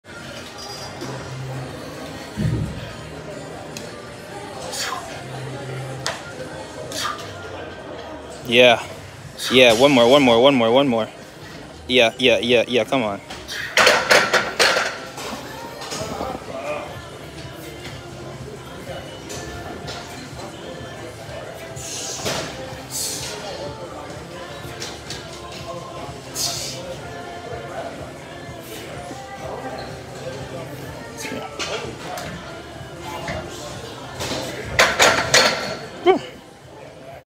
Seated OHP: 225x4 and 190x12
190x12 came first, it was the last set of my pyramid. I did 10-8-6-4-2-AMRAP with 190. Then, I threw 225 on and tried to rep it out for fun, the gamble paid off nicely